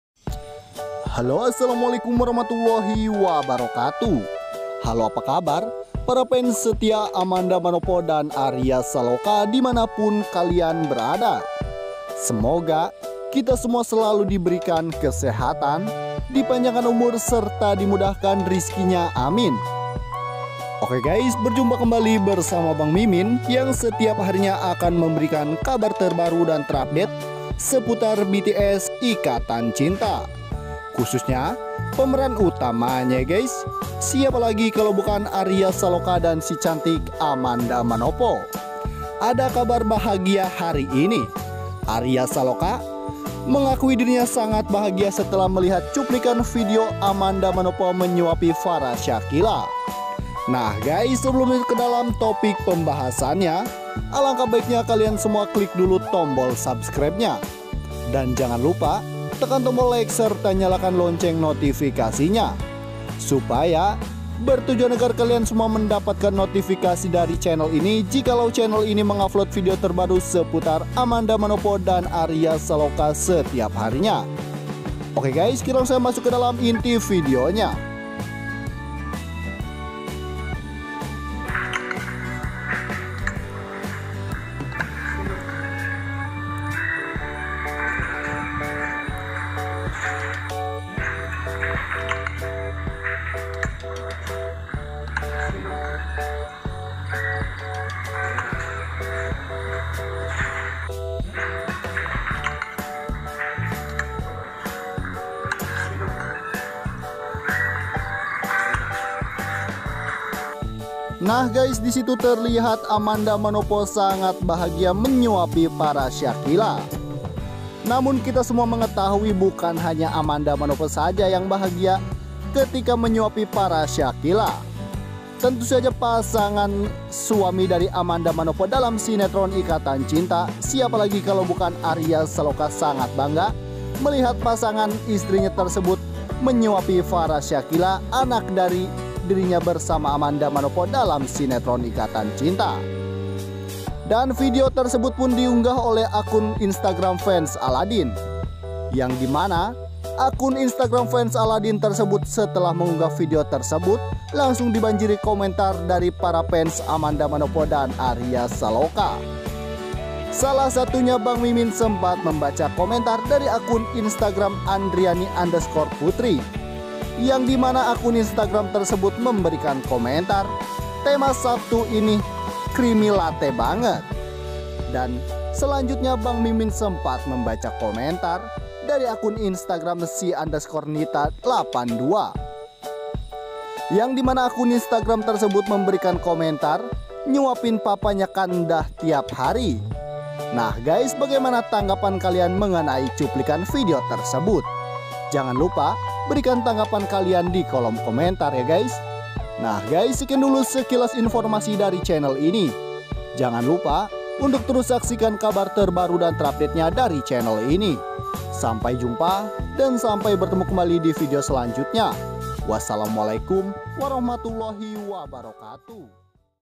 SOSWEET...!!! ARYA SALOKA AKUI DIRINYA BANGGA MELIHAT AMANDA MANOPO SUAPIN FARA SHAKILA MAKAN
__SEKIAN & TERIMAKASIH___